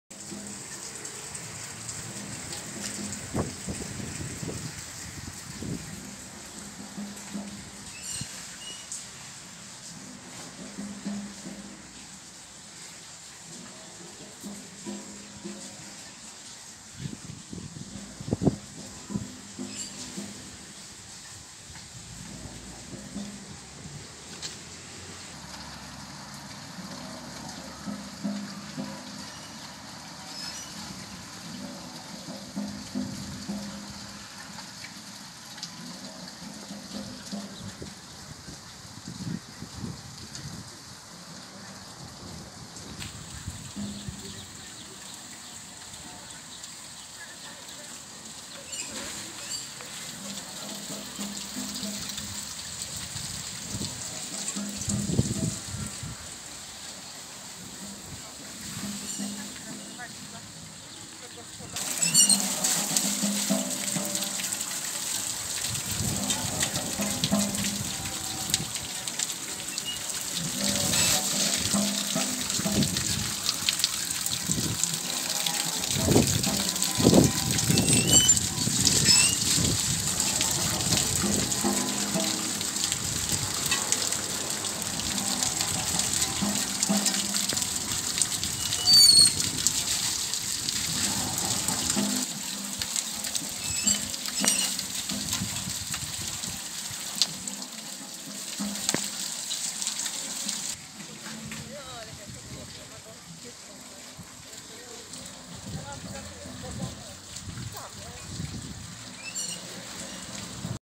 Skwer Reginy Osowickiej
Skwer koło Filharmonii Kaszubskiej przy ulicy Sobieskiego. Na skwerze znajduje się ławeczka z rzeźbą Pani Reginy Osowickiej - publicystki, działaczki regionalnej, popularyzatorki wiedzy o zbrodni w Piaśnicy. Jest tu także fontanna i kilka ciekawych donic. Sama Reginy uczyła w szkole znajdującej się obok.